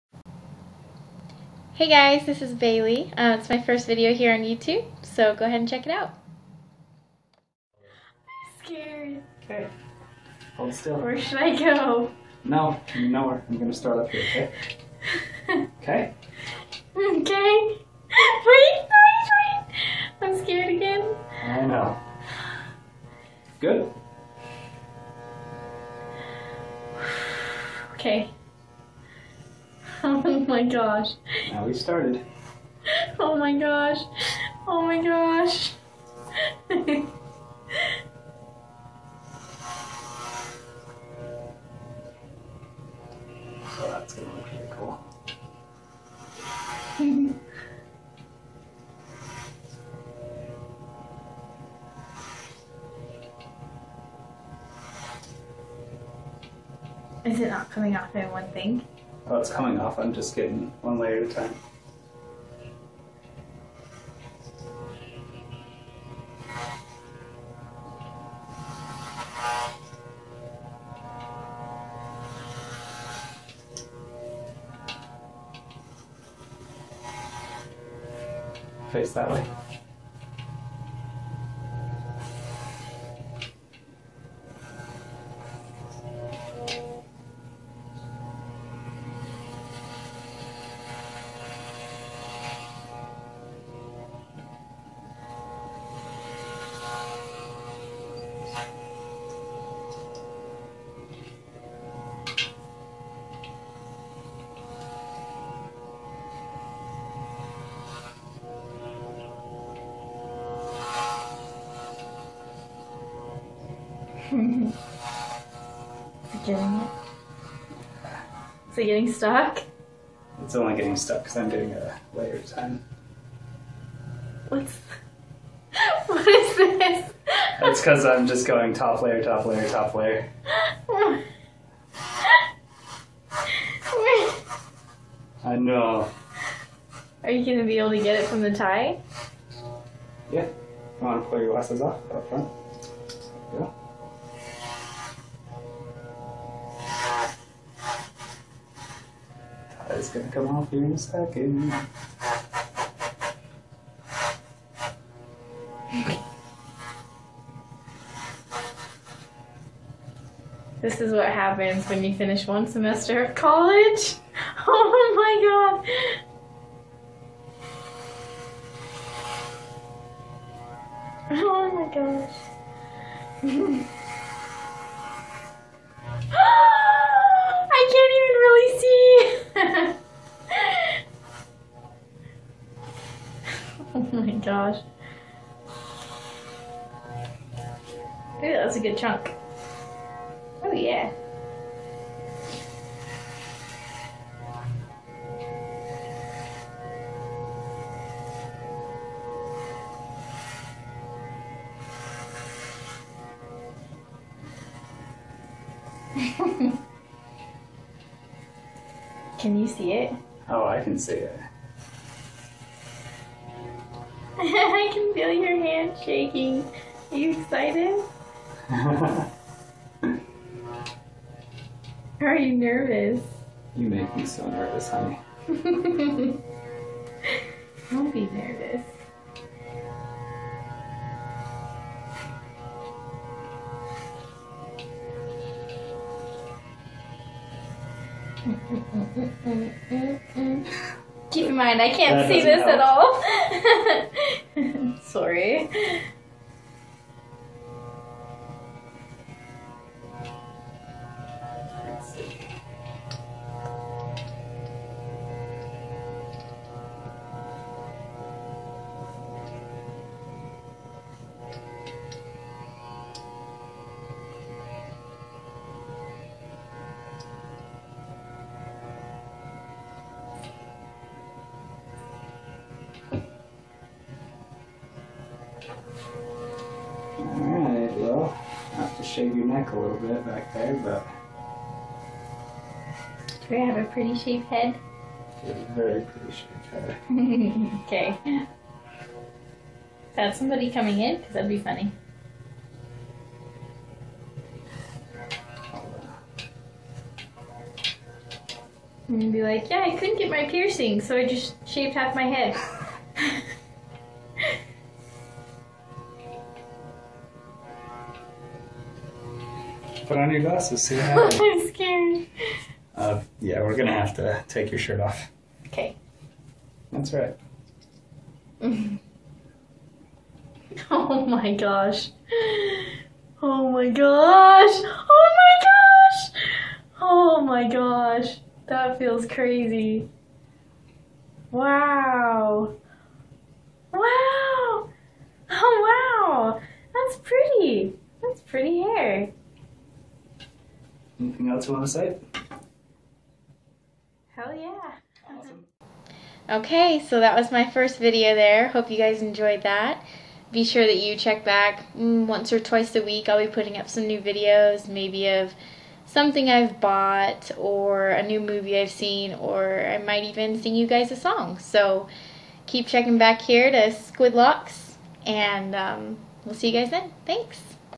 001 ~ I shaved my head into a Sidecut !! ~
This is me getting my head shaved yesterday for the first time! Feels so awesome! You should try it!! =)

Feel free to follow me or heart my pics! =)